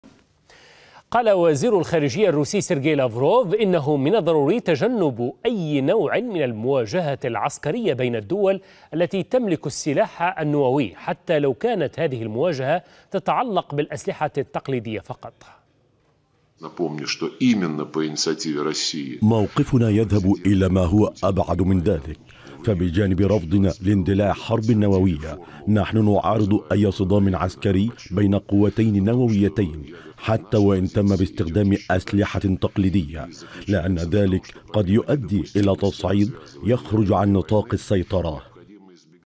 لافروف: يجب تجنب أي نوع من المواجهة العسكرية بين الدول التي تمتلك أسلحة نووية
قال وزير الخارجية الروسي سيرغي لافروف إنه من الضروري تجنب أي نوع من المواجهة العسكرية بين الدول التي تملك السلاح النووي، حتى لو كانت هذه المواجهة تتعلق بالأسلحة التقليدية فقط.